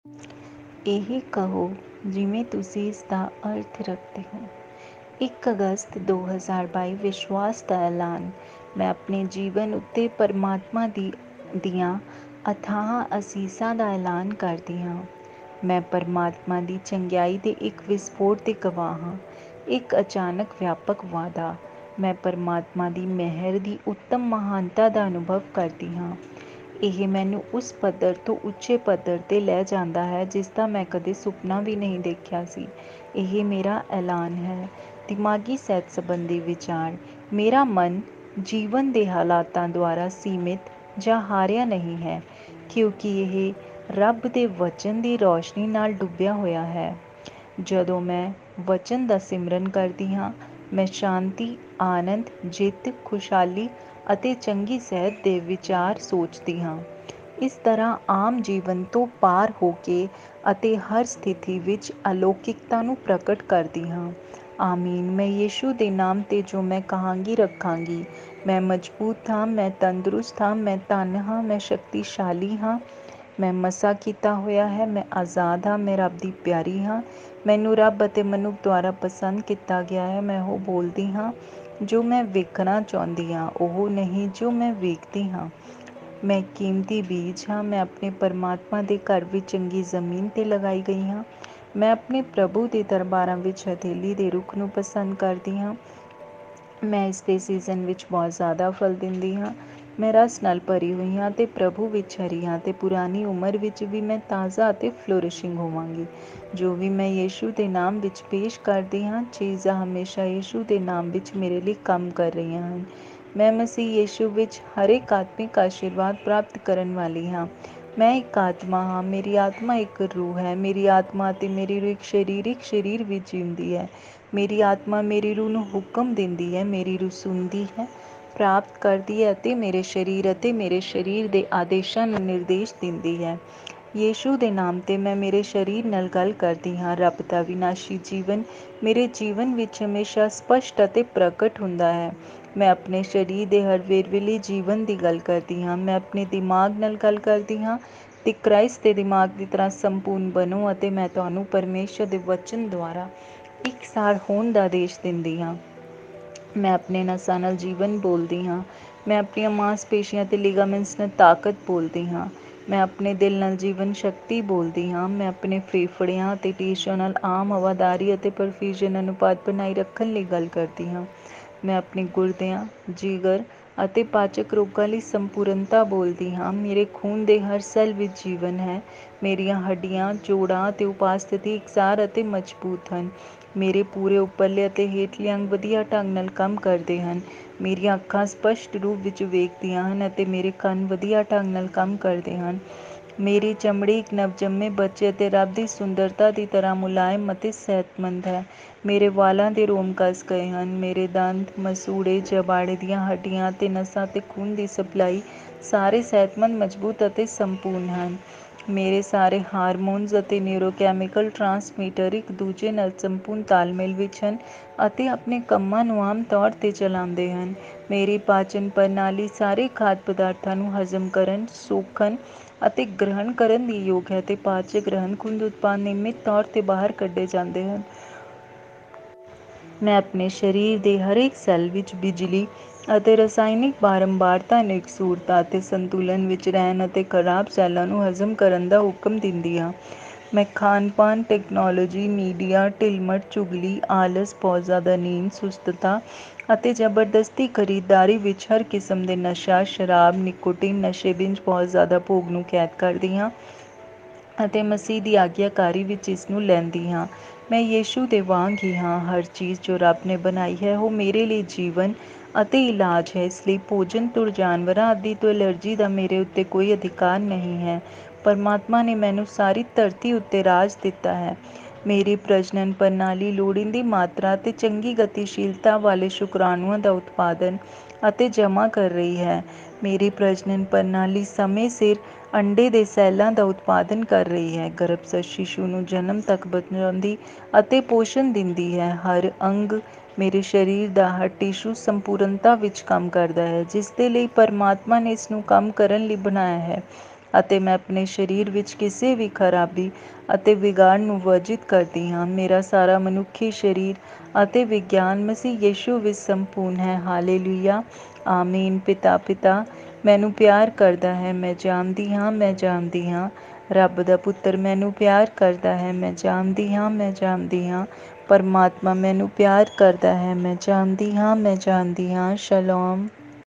1st August 2022 Say It like You Mean It (Audio in Punjabi)
ਇਹ ਕਹੋ ਜਿਵੇਂ ਤੁਸੀਂ ਇਸਦਾ ਅਰਥ ਰੱਖਦੇ ਹੋ
 1 ਅਗਸਤ 2022

 ਵਿਸ਼ਵਾਸ ਦੇ ਐਲਾਨ

 ਮੈਂ ਆਪਣੇ ਜੀਵਨ ਉੱਤੇ ਪ੍ਰਮਾਤਮਾ ਦੀਆਂ ਅਥਾਹ ਅਸੀਸਾਂ ਦਾ ਐਲਾਨ ਕਰਦੀ ਹਾਂ।  ਮੈਂ ਪ੍ਰਮਾਤਮਾ ਦੀ ਚੰਗਿਆਈ ਦੇ ਇੱਕ ਵਿਸਫੋਟ ਦੀ ਗਵਾਹ ਹਾਂ, ਇੱਕ ਅਚਾਨਕ ਵਿਆਪਕ ਵਾਧਾ।  ਮੈਂ ਪ੍ਰਮਾਤਮਾ ਦੀ ਮਿਹਰ ਦੀ ਉੱਤਮ ਮਹਾਨਤਾ ਦਾ ਅਨੁਭਵ ਕਰਦਾ ਹਾਂ। ਇਹ ਮੈਨੂੰ ਉਸ ਪੱਧਰ ਤੋਂ ਉੱਚੇ ਪੱਧਰ 'ਤੇ ਲੈ ਜਾਂਦਾ ਹੈ ਜਿਸਦਾ ਮੈਂ ਕਦੇ ਸੁਪਨਾ ਵੀ ਨਹੀਂ ਦੇਖਿਆ ਸੀ।  ਇਹ ਮੇਰਾ ਐਲਾਨ ਹੈ।

ਦਿਮਾਗੀ ਸਿਹਤ ਸੰਬੰਧੀ ਵਿਚਾਰ

 ਮੇਰਾ ਮਨ ਜੀਵਨ ਦੇ ਹਾਲਾਤਾਂ ਦੁਆਰਾ ਸੀਮਿਤ ਜਾਂ ਹਾਰਿਆ ਨਹੀਂ ਹੈ ਕਿਉਂਕਿ ਇਹ ਰੱਬ ਦੇ ਬਚਨ ਦੀ ਰੋਸ਼ਨੀ ਨਾਲ ਡੁੱਬਿਆ ਹੋਇਆ ਹੈ (ਹੜ੍ਹ)।  ਜਦੋਂ ਮੈਂ ਬਚਨ ਦਾ ਸਿਮਰਨ ਕਰਦੀ ਹਾਂ, ਮੈਂ ਸ਼ਾਂਤੀ, ਅਨੰਦ, ਜਿੱਤ, ਖੁਸ਼ਹਾਲੀ ਅਤੇ ਚੰਗੀ ਸਿਹਤ ਦੇ ਵਿਚਾਰ ਸੋਚਦੀ ਹਾਂ, ਇਸ ਤਰ੍ਹਾਂ, ਆਮ ਜੀਵਨ ਤੋਂ ਪਾਰ ਹੋ ਕੇ ਅਤੇ ਹਰ ਸਥਿਤੀ ਵਿੱਚ ਅਲੌਕਿਕਤਾ ਨੂੰ ਪ੍ਰਗਟ ਕਰਦੀ ਹਾਂ ਆਮੀਨ
ਮੈਂ ਯਿਸੂ ਦੇ ਨਾਮ ਤੇ, ਜੋ ਮੈਂ ਕਹਾਂਗੀ ,ਰੱਖਾਂਗੀ(ਜੌਨ14:13)
 ਮੈਂ ਮਜ਼ਬੂਤ ​​ਹਾਂ।(ਐਫ਼ 6:10)
 ਮੈਂ ਤੰਦਰੁਸਤ ਹਾਂ। (1 ਪਤ 2:24)
 ਮੈਂ ਧੰਨ ਹਾਂ।(ਅਫ਼ 1:3)
 ਮੈਂ ਸ਼ਕਤੀਸ਼ਾਲੀ ਹਾਂ। (ਇਸ 40:29)
 ਮੈਂ ਮਸਹ ਕੀਤਾ ਹੋਇਆ ਹਾਂ (1 ਜੌਨ 2:20)
 ਮੈਂ ਆਜਾਦ ਹਾਂ।(ਯੂਹੰਨਾ 8:36)
 ਮੈਂ ਰੱਬ ਦੀ ਪਿਆਰੀ ਹਾਂ।(ਮਰਕੁਸ 1:11)
 ਮੈਨੂੰ ਰੱਬ ਅਤੇ ਮਨੁੱਖ ਦੁਆਰਾ ਪਸੰਦ ਕੀਤਾ ਗਿਆ ਹੈ।(ਉਤਪਤ 39:2-3)
 ਮੈਂ ਉਹ ਬੋਲਦੀ ਹਾਂ ਜੋ ਮੈਂ ਵੇਖਣਾ ਚਾਹੁੰਦੀ ਹਾਂ ਉਹ ਨਹੀਂ ਜੋ ਮੈਂ ਵੇਖਦੀ ਹਾਂ ।(2 ਕੁਰਿੰਥੀਆਂ 4:18)
 ਮੈਂ ਇੱਕ ਕੀਮਤੀ ਬੀਜ ਹਾਂ। (ਲੂਕਾ 8:15)
 ਮੈਂ ਆਪਣੇ ਪਰਮਾਤਮਾ ਦੇ ਘਰ ਵਿੱਚ ਚੰਗੀ ਜ਼ਮੀਨ 'ਤੇ ਲਗਾਈ ਗਈ ਹਾਂ। (ਜ਼ਬੂਰ 92:11)
 ਮੈਂ ਆਪਣੇ ਪ੍ਰਭੂ ਦੇ ਦਰਬਾਰਾਂ ਵਿੱਚ ਹਥੇਲੀ ਦੇ ਰੁੱਖ ਨੂੰ ਪਸੰਦ ਕਰਦੀ ਹਾਂ।(ਜ਼ਬੂਰ 92:12)
 ਮੈਂ ਇਸ ਦੇ ਸੀਜ਼ਨ ਵਿੱਚ ਬਹੁਤ ਜ਼ਿਆਦਾ ਫਲ ਦਿੰਦੀ ਹਾਂ।(ਜ਼ਬੂਰ 1:3)
 ਮੈਂ ਰਸ ਨਾਲ ਭਰੀ ਹੋਈ ਹੈ ਅਤੇ ਪ੍ਰਭੂ ਵਿੱਚ  ਹਰੀ ਹਾਂ(ਜ਼ਬੂਰ 92:14)
 ਅਤੇ ਪੁਰਾਣੀ ਉਮਰ ਵਿੱਚ ਵੀ ਮੈਂ ਤਾਜ਼ਾ ਅਤੇ ਫਲੋਰਿਸ਼ਿੰਗ ਹੋਵੇਗੀ ।(ਜ਼ਬੂਰ 92:14)
 ਜੋ ਵੀ ਮੈਂ ਯਿਸੂ ਦੇ ਨਾਮ ਵਿੱਚ ਪੇਸ਼ ਕਰਦੀ ਹਾਂ।(ਜ਼ਬੂਰ1:3)
 ਚੀਜ਼ਾਂ ਹਮੇਸ਼ਾਂ ਯਿਸੂ ਦੇ ਨਾਮ ਵਿੱਚ ਮੇਰੇ ਲਈ ਕੰਮ ਕਰ ਰਹੀਆਂ ਹਨ(ਰੋਮ 8:28)
 ਮੈਂ ਮਸੀਹ ਯਿਸੂ ਵਿੱਚ ਹਰ ਇੱਕ ਆਤਮਿਕ ਆਸ਼ੀਰਵਾਦ ਪ੍ਰਾਪਤ ਕਰਨ ਵਾਲੀ ਹਾਂ।(ਅਫ਼ 1:3)

 ਮੈਂ ਇੱਕ ਆਤਮਾ ਹਾਂ
 ਮੇਰੀ ਆਤਮਾ ਇੱਕ ਰੂਹ ਹੈ
 ਮੇਰੀ ਆਤਮਾ ਅਤੇ ਮੇਰੀ ਰੂਹ ਇੱਕ ਸਰੀਰਕ ਸਰੀਰ ਵਿੱਚ ਜੀਉਂਦੀ ਹੈ
 ਮੇਰੀ ਆਤਮਾ ਮੇਰੀ ਰੂਹ ਨੂੰ ਹੁਕਮ ਦਿੰਦੀ ਹੈ।ਮੇਰੀ ਰੂਹ ਸੁਣਦੀ ਹੈ, ਪ੍ਰਾਪਤ ਕਰਦੀ ਹੈ ਅਤੇ ਮੇਰੇ ਸਰੀਰ ਅਤੇ ਮੇਰੇ ਸਰੀਰ ਦੇ ਆਦੇਸ਼ਾਂ ਨੂੰ ਨਿਰਦੇਸ਼ ਦਿੰਦੀ ਹੈ

 ਯਿਸੂ ਦੇ ਨਾਮ ਤੇ, ਮੈਂ ਮੇਰੇ ਸਰੀਰ ਨਾਲ ਗੱਲ ਕਰਦੀ ਹਾਂ
 ਰੱਬ ਦਾ ਅਵਿਨਾਸ਼ੀ ਜੀਵਨ ਮੇਰੇ ਜੀਵਨ ਵਿੱਚ ਹਮੇਸ਼ਾਂ ਸਪਸ਼ਟ ਅਤੇ ਪ੍ਰਗਟ ਹੁੰਦਾ ਹੈ
 ⚓ਮੈਂ ਆਪਣੇ ਸਰੀਰ ਦੇ ਹਰ ਵੇਰਵੇ ਲਈ ਜੀਵਨ ਦੀ ਗੱਲ ਕਰਦੀ ਹਾਂ।
⚓ਮੈਂ ਆਪਣੇ ਦਿਮਾਗ ਨਾਲ ਗੱਲ ਕਰਦੀ ਹਾਂ ।ਦਿ ਕਰਾਇਸਟ ਦੇ ਦਿਮਾਗ ਦੀ ਤਰ੍ਹਾਂ ਸੰਪੂਰਨ ਬਣੋ ਅਤੇ ਮੈਂ ਤੁਹਾਨੂੰ ਪਰਮੇਸ਼ੁਰ ਦੇ ਬਚਨ ਦੁਆਰਾ  ਇਕਸਾਰ  ਹੋਣ ਦਾ ਆਦੇਸ਼ ਦਿੰਦੀ ਹਾਂ।
⚓ਮੈਂ ਆਪਣੇ ਨਸਾਂ ਨਾਲ  ਜੀਵਨ ਬੋਲਦੀ ਹਾਂ।
⚓ਮੈਂ ਆਪਣੀਆਂ ਮਾਸਪੇਸ਼ੀਆਂ ਅਤੇ ਲਿਗਾਮੈਂਟਸ ਨਾਲ ਤਾਕਤ ਬੋਲਦੀ ਹਾਂ।
⚓ਮੈਂ ਆਪਣੇ ਦਿਲ ਨਾਲ  ਜੀਵਨਸ਼ਕਤੀ ਬੋਲਦੀ ਹਾਂ।
⚓ਮੈਂ ਆਪਣੇ ਫੇਫੜਿਆਂ ਅਤੇ ਟਿਸ਼ੂਆਂ ਨਾਲ ਆਮ ਹਵਾਦਾਰੀ ਅਤੇ ਪਰਫਿusionਜ਼ਨ ਅਨੁਪਾਤ ਬਣਾਈ ਰੱਖਣ ਲਈ ਗੱਲ ਕਰਦੀ ਹਾਂ।
⚓ਮੈਂ ਆਪਣੇ ਗੁਰਦਿਆਂ, ਜਿਗਰ ਅਤੇ ਪਾਚਕ ਰੋਗਾਂ ਲਈ  ਸੰਪੂਰਨਤਾ  ਬੋਲਦੀ ਹਾਂ।
⚓ਮੇਰੇ ਖੂਨ ਦੇ ਹਰ ਸੈੱਲ ਵਿੱਚ ਜੀਵਨ ਹੈ ਮੇਰੀਆਂ ਹੱਡੀਆਂ, ਜੋੜਾਂ ਅਤੇ ਉਪਾਸਥੀ ,ਇਕਸਾਰ, ​​ਅਤੇ ਮਜ਼ਬੂਤ ​​ਹਨ ।
⚓ਮੇਰੇ ਪੂਰੇ ਉਪਰਲੇ ਅਤੇ ਹੇਠਲੇ ਅੰਗ ਵਧੀਆ ਢੰਗ ਨਾਲ ਕੰਮ ਕਰਦੇ ਹਨ ।
⚓ਮੇਰੀਆਂ ਅੱਖਾਂ ਸਪਸ਼ਟ ਰੂਪ ਵਿੱਚ ਵੇਖਦੀਆਂ ਹਨ  ਅਤੇ
⚓ਮੇਰੇ ਕੰਨ ਵਧੀਆ *ੰਗ  ਨਾਲ ਕੰਮ ਕਰਦੇ ਹਨ । ਮੇਰੀ ਚਮੜੀ ਇੱਕ ਨਵਜੰਮੇ ਬੱਚੇ ਅਤੇ ਰੱਬ ਦੀ ਸੁੰਦਰਤਾ ਦੀ ਤਰ੍ਹਾਂ  ਮੁਲਾਇਮ ਅਤੇ ਸਿਹਤਮੰਦ ਹੈ।
⚓ਮੇਰੇ ਵਾਲਾਂ ਦੇ ਰੋਮ ਕੱਸ ਗਏ ਹਨ।
⚓ਮੇਰੇ ਦੰਦ, ਮਸੂੜੇ, ਜਬਾੜੇ ਦੀਆਂ ਹੱਡੀਆਂ ਅਤੇ ਨਸਾਂ ਅਤੇ ਖੂਨ ਦੀ ਸਪਲਾਈ ਸਾਰੇ ਸਿਹਤਮੰਦ, ਮਜ਼ਬੂਤ ​​ਅਤੇ ਸੰਪੂਰਨ ਹਨ
⚓ਮੇਰੇ ਸਾਰੇ ਹਾਰਮੋਨਸ ਅਤੇ ਨਿਰੋਕੈਮੀਕਲ ਟ੍ਰਾਂਸਮੀਟਰ ਇੱਕ ਦੂਜੇ ਨਾਲ ਸੰਪੂਰਨ ਤਾਲਮੇਲ ਵਿੱਚ ਹਨ ਅਤੇ ਆਪਣੇ ਕੰਮਾਂ ਨੂੰ ਆਮ ਤੌਰ ਤੇ ਚਲਾਉਂਦੇ ਹਨ ।
⚓ਮੇਰੀ ਪਾਚਨ ਪ੍ਰਣਾਲੀ ਸਾਰੇ ਖਾਧ ਪਦਾਰਥਾਂ ਨੂੰ ਹਜ਼ਮ ਕਰਨ, ਸੋਖਣ ਅਤੇ ਗ੍ਰਹਿਣ ਕਰਨ ਦੇ ਯੋਗ ਹੈ ਅਤੇ ਪਾਚਕ ਰਹਿੰਦ -ਖੂੰਹਦ ਉਤਪਾਦ ਨਿਯਮਤ ਤੌਰ ਤੇ ਬਾਹਰ ਕੱੇ ਜਾਂਦੇ ਹਨ ।
⚓ ਮੈਂ ਆਪਣੇ ਸਰੀਰ ਦੇ ਹਰੇਕ ਸੈੱਲ ਵਿੱਚ ਬਿਜਲੀ ਅਤੇ ਰਸਾਇਣਕ ਬਾਰੰਬਾਰਤਾ ਨੂੰ ਇਕਸੁਰਤਾ ਅਤੇ ਸੰਤੁਲਨ ਵਿੱਚ ਰਹਿਣ ਅਤੇ ਖਰਾਬ ਸੈੱਲਾਂ ਨੂੰ ਹਜ਼ਮ ਕਰਨ ਦਾ ਹੁਕਮ ਦਿੰਦੀ ਹਾਂ।
⚓ਮੈਂ ਖਾਣ-ਪੀਣ, ਤਕਨਾਲੋਜੀ, ਮੀਡੀਆ, ਢਿੱਲ-ਮੱਠ, ਚੁਗਲੀ, ਆਲਸ, ਬਹੁਤ ਜ਼ਿਆਦਾ ਨੀਂਦ, ਸੁਸਤਤਾ, ਅਤੇ ਜਬਰਦਸਤੀ ਖਰੀਦਦਾਰੀ ਵਿੱਚ ਹਰ ਕਿਸਮ ਦੇ ਨਸ਼ਾ (ਸ਼ਰਾਬ, ਨਿਕੋਟੀਨ, ਨਸ਼ੇ) ਬਿੰਜ (ਬਹੁਤ ਜ਼ਿਆਦਾ ਭੋਗ) ਨੂੰ ਕੈਦ ਕਰਦੀ ਹਾਂ ਅਤੇ ਮਸੀਹ ਦੀ ਆਗਿਆਕਾਰੀ ਵਿੱਚ ਇਸਨੂੰ ਲੈਂਦੀ ਹਾਂ। ਮੈਂ ਯਿਸੂ ਵਾਂਗ ਹੀ ਹਾਂ।
⚓ਹਰ ਚੀਜ਼ ਜੋ ਰੱਬ ਨੇ ਬਣਾਈ ਹੈ ਉਹ ਮੇਰੇ ਲਈ ਜੀਵਨ ਅਤੇ ਇਲਾਜ ਹੈ, ਇਸਲਈ ਭੋਜਨ, ਧੂੜ, ਜਾਨਵਰਾਂ ਆਦਿ ਤੋਂ ਐਲਰਜੀ ਦਾ ਮੇਰੇ ਉੱਤੇ ਕੋਈ ਅਧਿਕਾਰ ਨਹੀਂ ਹੈ। ਪਰਮਾਤਮਾ ਨੇ ਮੈਨੂੰ ਸਾਰੀ ਧਰਤੀ ਉੱਤੇ ਰਾਜ ਦਿੱਤਾ ਹੈ
⚓ 🙎🏻‍♂️ ♂️ਮੇਰੀ ਪ੍ਰਜਨਨ ਪ੍ਰਣਾਲੀ ਲੋੜੀਂਦੀ ਮਾਤਰਾ ਅਤੇ ਚੰਗੀ ਗਤੀਸ਼ੀਲਤਾ ਵਾਲੇ ਸ਼ੁਕਰਾਣੂਆਂ ਦਾ ਉਤਪਾਦਨ ਅਤੇ ਜਮ੍ਹਾਂ ਕਰ ਰਹੀ ਹੈ
⚓ 🙍🏻‍♀️ ‍♀️ਮੇਰੀ ਪ੍ਰਜਨਨ ਪ੍ਰਣਾਲੀ ਸਮੇਂ ਸਿਰ ਅੰਡੇ ਦੇ ਸੈੱਲਾਂ ਦਾ ਉਤਪਾਦਨ ਕਰ ਰਹੀ ਹੈ, ਗਰੱਭਸਥ ਸ਼ੀਸ਼ੂ ਨੂੰ ਜਨਮ  ਤੱਕ ਬਚਾਉਂਦੀ ਅਤੇ ਪੋਸ਼ਣ ਦਿੰਦੀ ਹੈ ।
⚓  ਹਰ ਅੰਗ, ਮੇਰੇ ਸਰੀਰ ਦਾ ਹਰ ਟਿਸ਼ੂ ਸੰਪੂਰਨਤਾ ਵਿੱਚ ਕੰਮ ਕਰਦਾ ਹੈ । ਜਿਸਦੇ ਲਈ ਪਰਮਾਤਮਾ ਨੇ ਇਸਨੂੰ ਕੰਮ ਕਰਨ ਲਈ ਬਣਾਇਆ ਹੈ, ਅਤੇ ਮੈਂ ਆਪਣੇ ਸਰੀਰ ਵਿੱਚ ਕਿਸੇ ਵੀ ਖਰਾਬੀ ਅਤੇ ਵਿਗਾੜ ਨੂੰ ਵਰਜਿਤ ਕਰਦੀ ਹਾਂ।
⚓  ਮੇਰਾ ਸਾਰਾ ਮਨੁੱਖੀ ਸਰੀਰ ਅਤੇ ਵਿਗਿਆਨ ਮਸੀਹ ਯਿਸੂ ਵਿੱਚ ਸੰਪੂਰਨ ਹੈ।ਹਲਲੂਯਾਹ! ਆਮੀਨ

 ਪਿਤਾ ਪਿਤਾ, ਮੈਨੂੰ ਪਿਆਰ ਕਰਦਾ ਹੈ, ਮੈਂ  ਜਾਣਦੀ ਹਾਂ ਮੈਂ ਜਾਣਦੀ ਹਾਂ
 ਰੱਬ ਦਾ ਪੁੱਤਰ, ਮੈਨੂੰ ਪਿਆਰ ਕਰਦਾ ਹੈ, ਮੈਂ ਜਾਣਦੀ ਹਾਂ ਮੈਂ ਜਾਣਦੀ ਹਾਂ
 ਪ੍ਰਮਾਤਮਾ ਮੈਨੂੰ ਪਿਆਰ ਕਰਦਾ ਹੈ, ਮੈਂ  ਜਾਣਦੀ ਹਾਂ ਮੈਂ ਜਾਣਦੀ ਹਾਂ

 ਸ਼ਲੋਮ